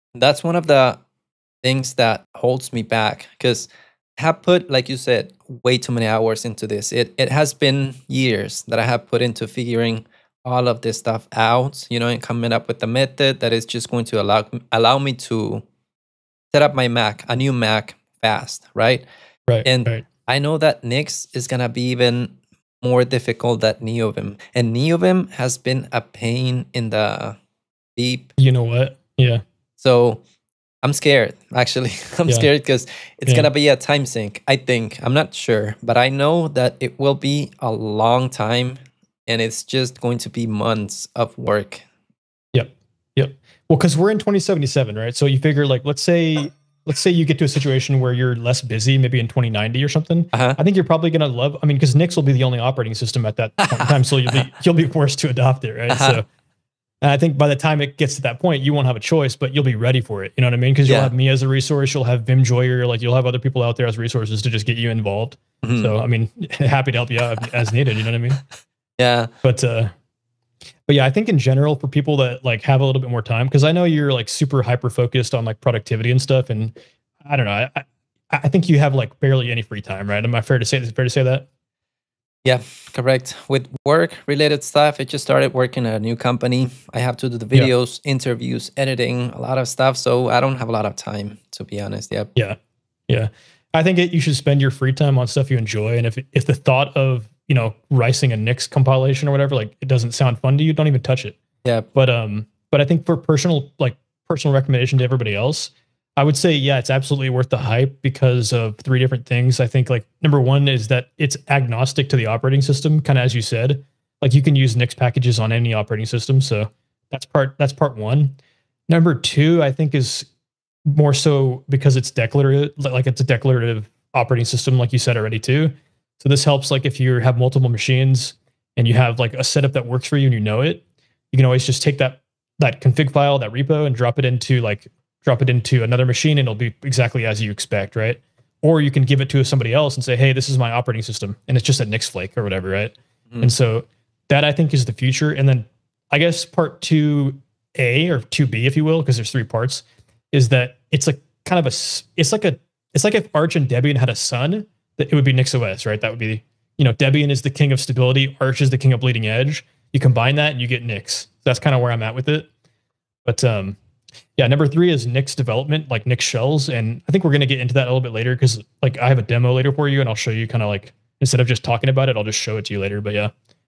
Should I go for Nix?
Clip of the interview with  @tony-btw  should I be scared of nix, is it going to require a lot if time?